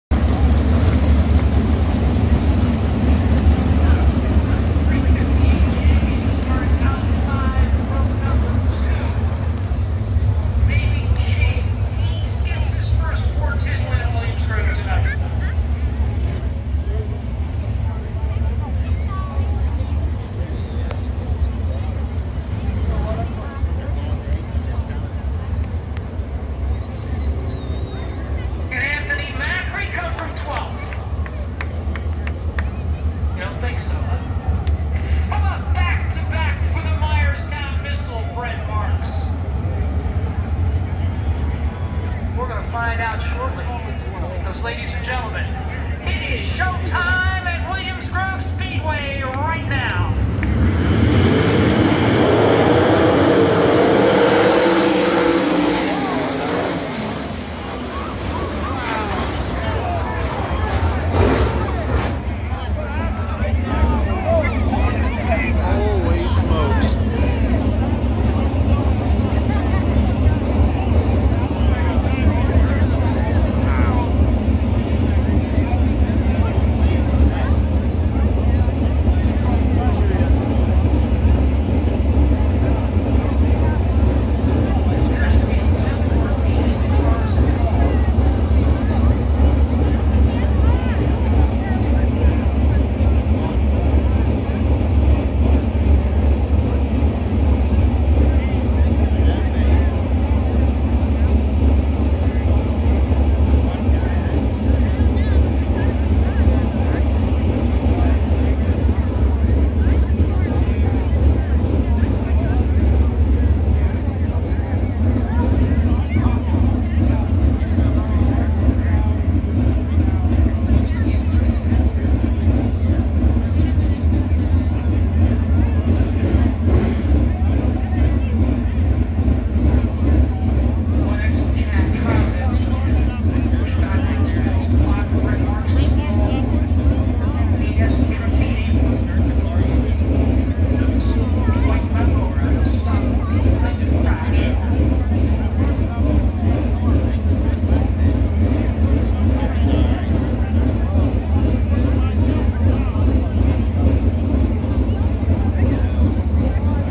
Massive Crash during the start of the 410 Sprints @ Williams Grove 8/21/2020
Crash occurred at the start of the 410 Sprint Feature at Williams Grove Speedway on August 21, 2020.